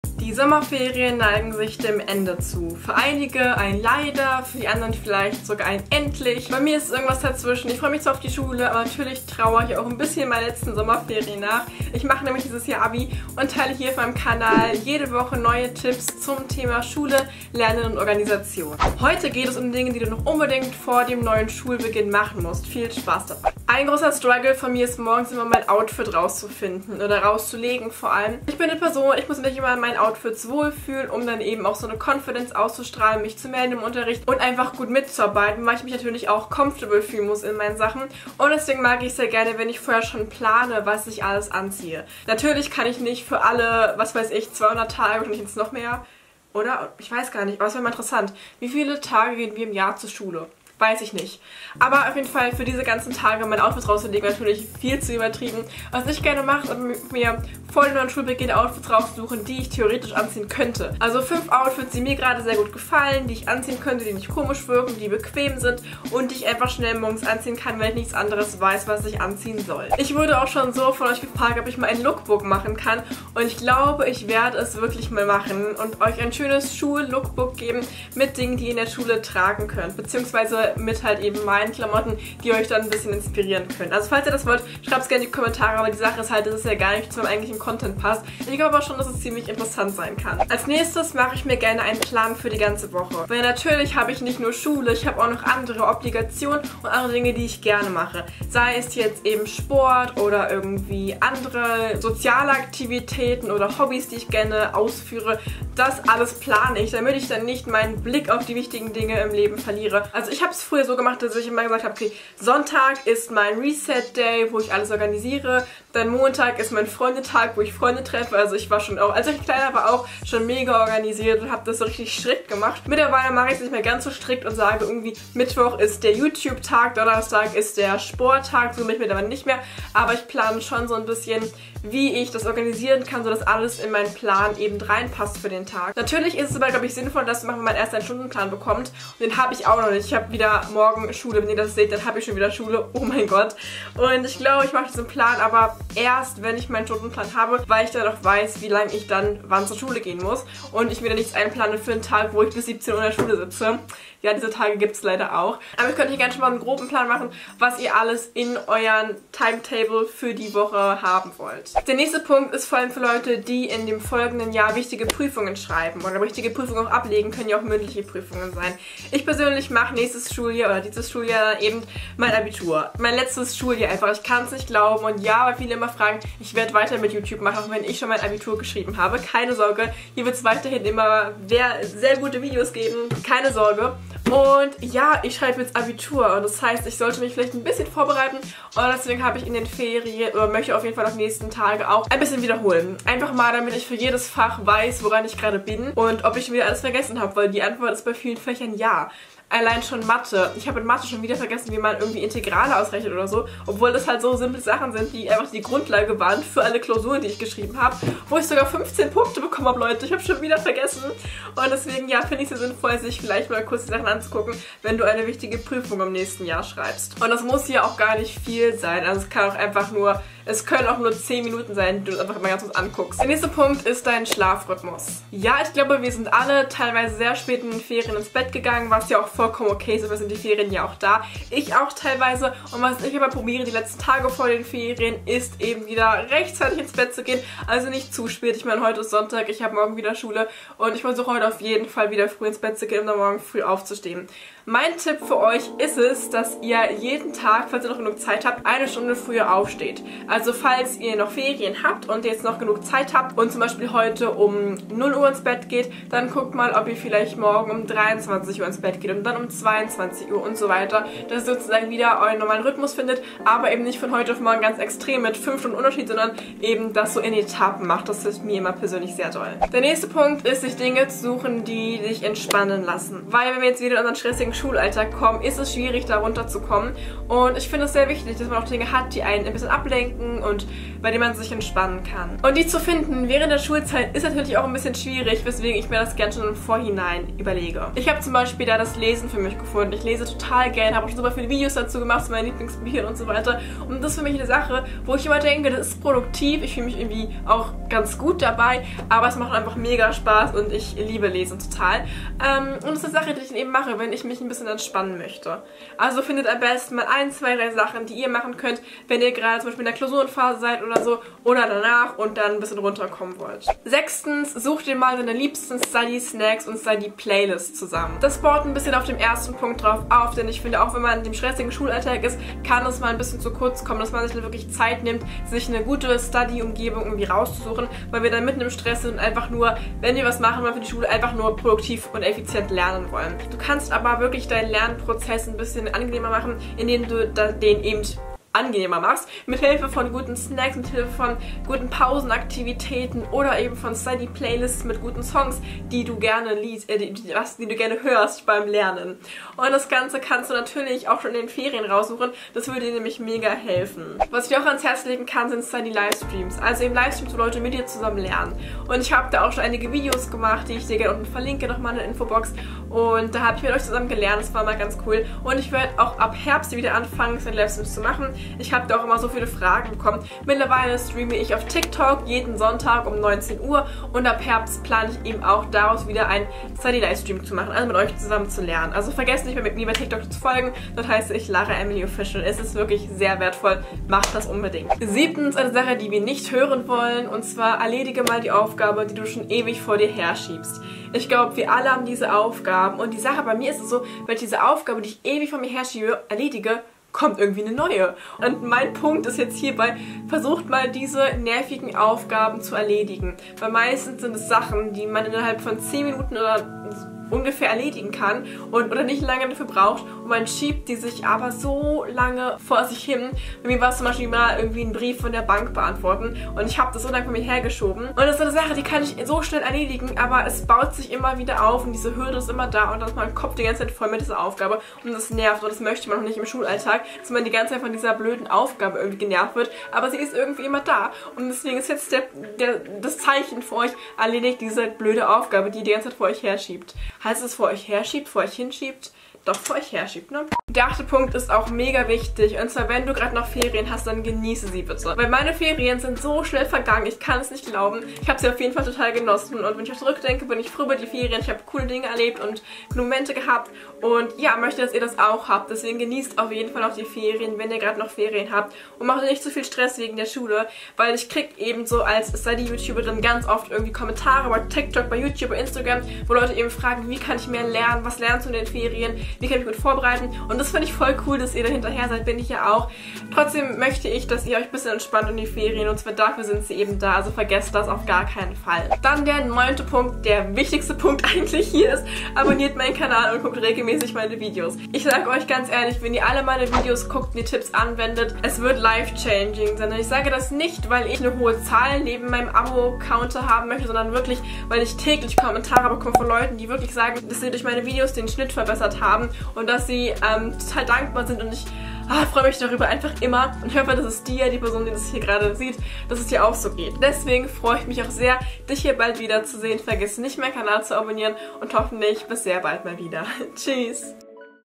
9 Dinge, die du am Anfang des Schuljahres machen solltest | Back To School
Ein neues Back To School Video! Heute geht es um die optimale Vorbereitung für das neue Schuljahr ✨

PODCAST: Gut Genug - Lara Emily

Ich spreche über meine Erfahrungen mit Produktivität, Organisation, Veganismus und Lernmethoden. Wie schön, dass du ein Teil davon bist!❤️